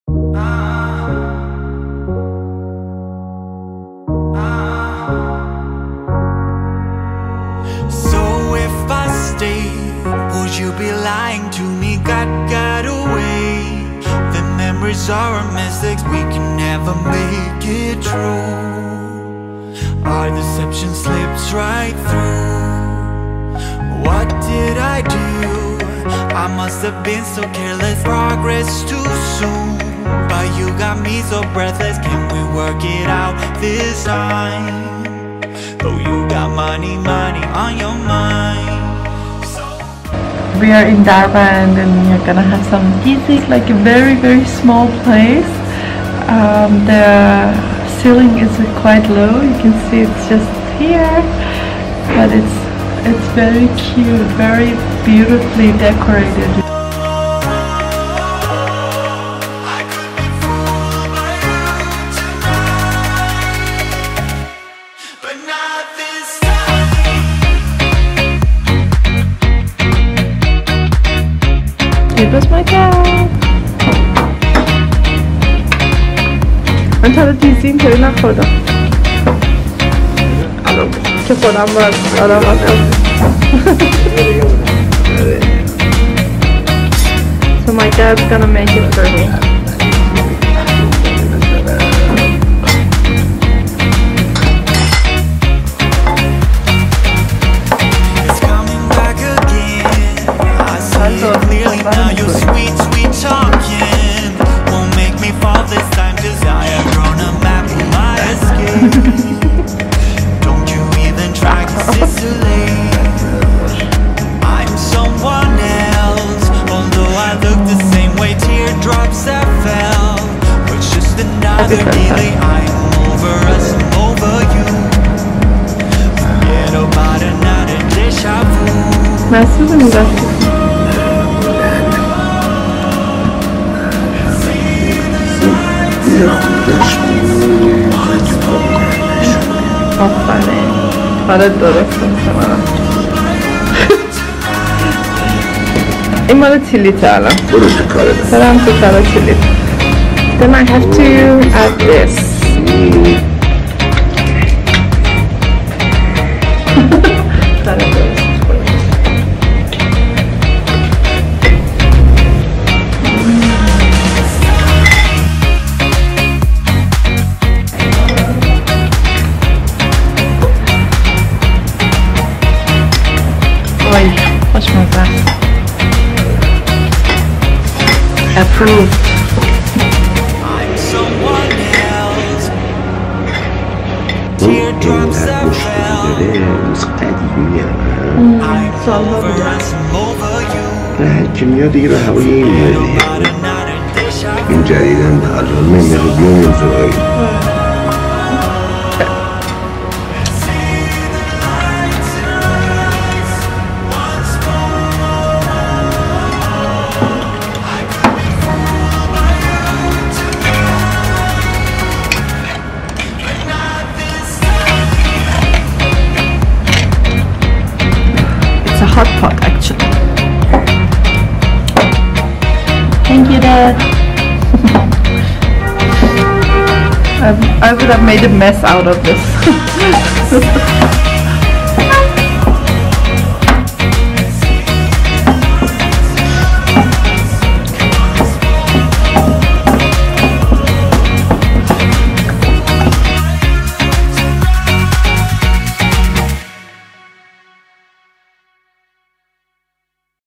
IRAN VLOG 2019: Having Dizi With My Dad - دیزی
When I was in Iran last year, one day, after doing some paperwork, my dad and I went to north of Tehran, Darband, to have some traditional Persian food. The place that my dad usually goes to was closed so we tried this one. But it was delicious that my dad said he will start coming here from then on.